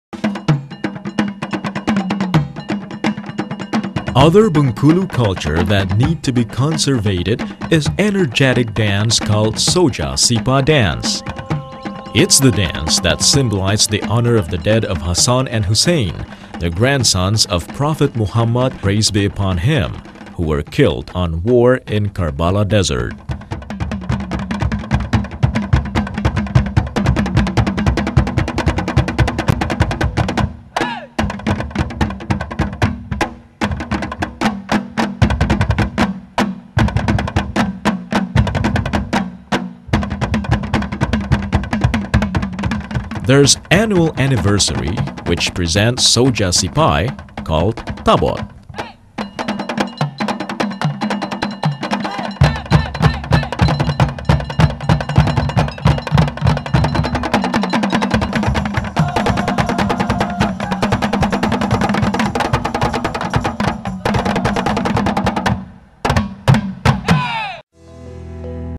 Soja Sipai Dance from Bengkulu, Indonesia (In English)
Soja Sipai Dance is a dance presented to Imam Hussein who was died in Karballa (Iraq).